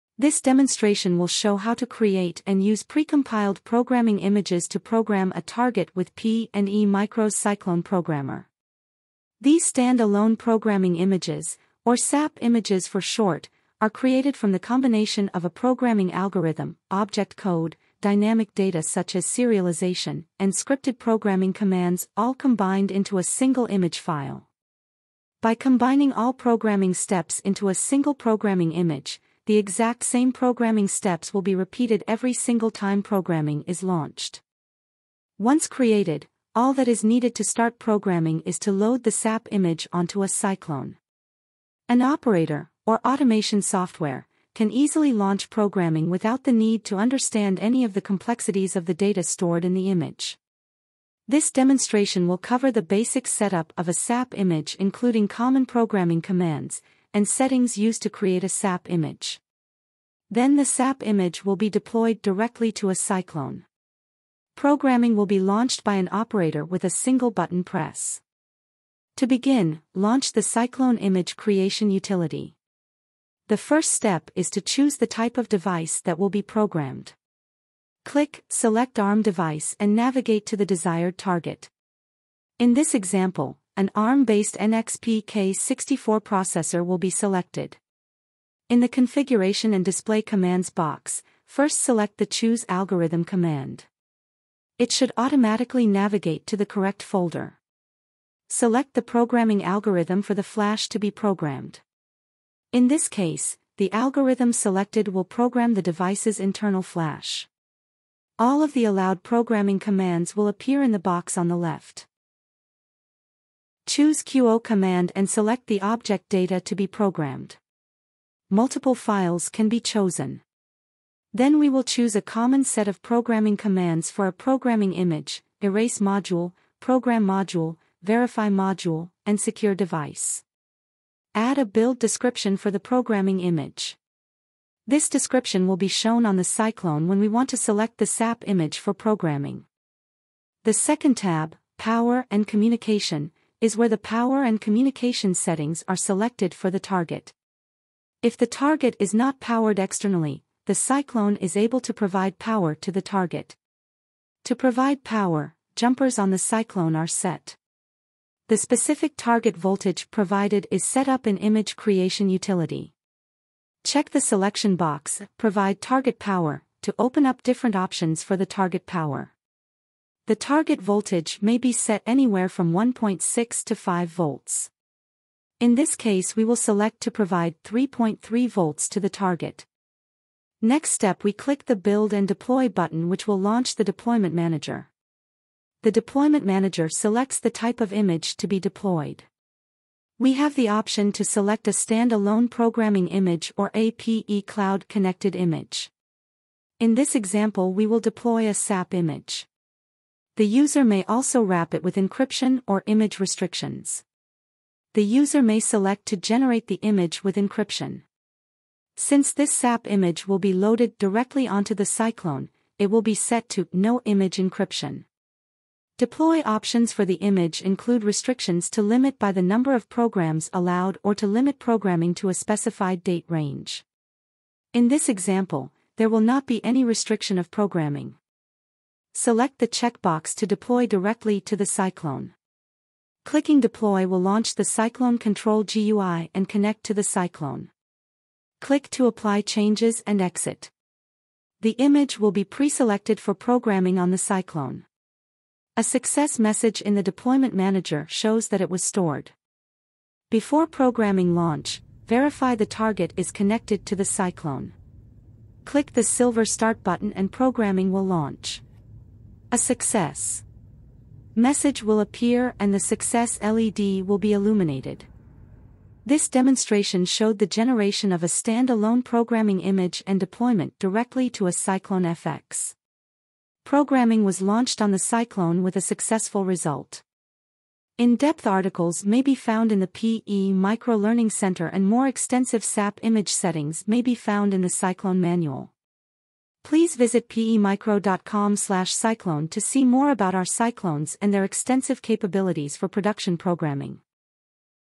PEmicro Cyclone Tutorial: Generation of a Stand-Alone Programming Image
PEmicro's Cyclone in-system programmers use stand-alone programming (SAP) images that are created by the user. This video provides an overview of what constitutes a programming image and demonstrates how to set up a basic SAP image using the Cyclone Image Creation Utility.

Topics covered during setup include:

- Target Power & Voltage Settings
- Communication Settings
- SAP image protection, including RSA/AES encryption that will only allow your SAP image to be decrypted on specific Cyclones

This video references the following links:

5 Ways To Power Your Target With the Cyclone Programmer